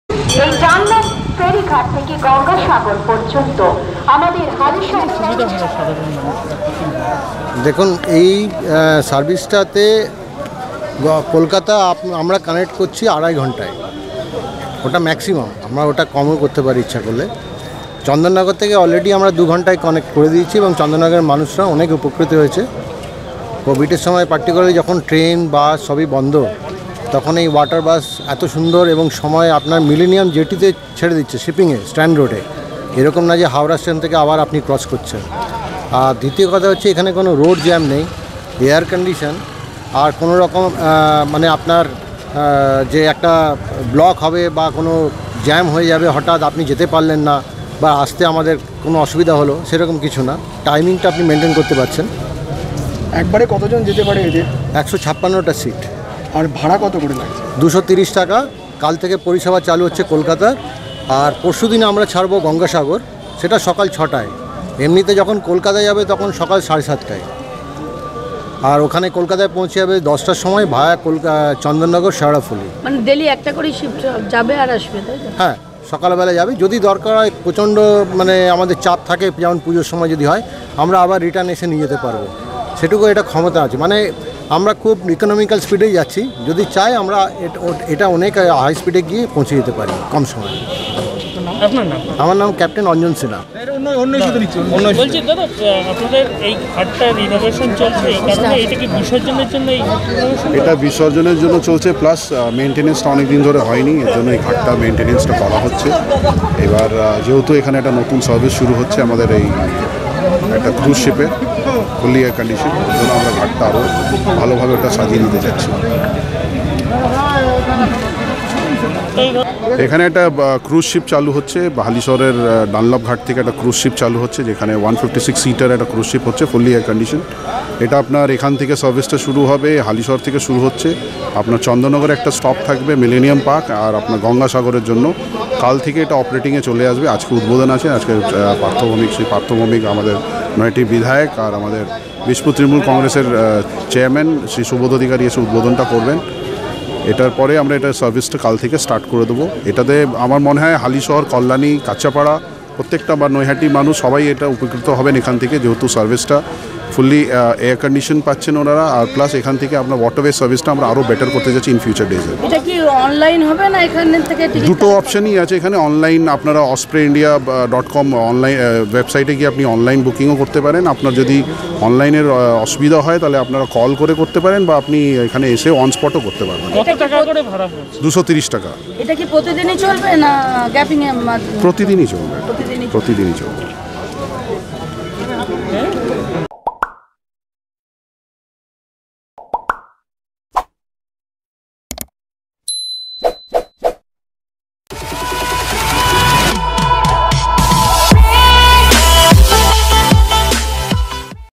হালিশহর ডানলপ ঘাট থেকে চালু হলো ক্রুজ শিপ লজ পরিষেবা | theregular.in
সমস্ত নিউজ আপডেট জানার জন্য চোখ রাখুন theregular.in এর পেজে | Get news update on theregular.in page | 20 Oct 2020

হালিশহর থেকে চালু হলো ক্রুজ শিপ লজ পরিষেবা এখন প্রচুর মানুষ এক জায়গা থেকে অন্য জায়গায় যেতে পারবে এই প্রথম হালিশহর পৌরসভার তৎপরতায় তৈরি করা হয়। আজ হালিশহরে ডানলপ ঘাটে এই অনুষ্ঠানের শুভ উদ্বোধন হয়।

বাইট হালিশহর চেয়ারম্যান রাজু সাহানি